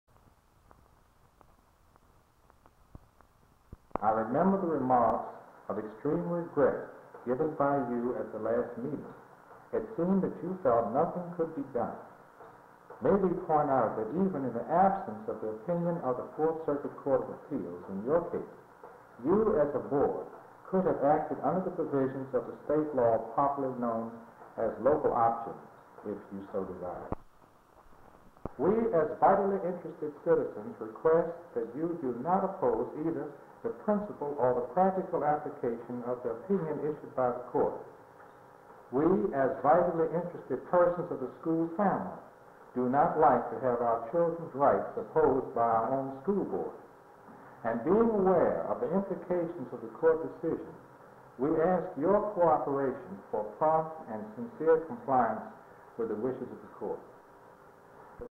Attorney George Lawrence (June 15, 1962)
Attorney George Lawrence Appeals to Officials for Better Conditions at Booker T. Washington High School

4353
(LIVE ON)
School Superintendent E.W. Rushton has been directed to set up a meeting between the Board and City Council to plan a course of action concerning the Recent US 4th Circuite Court of Appeals Decision. The Court, in effect, declared Roanokes method of assigning negro children to schools discriminatory and ordered the practice stopped. The School Board will also consult Council on the legality of Changing its "Housing Plan" to relieve crowded conditions at Booker T. Washington High School. For the second consecutive meeting, attorney George Lawrence, representing negro PTA groups, complained about conditions.
(ROLL FILM)
Lawrence couched his appeal for more seats with many references to the recent Court decision.
(TAKE FILM SOUND UP End Cue: "with the wishes of the Court".)
(LIVE ON)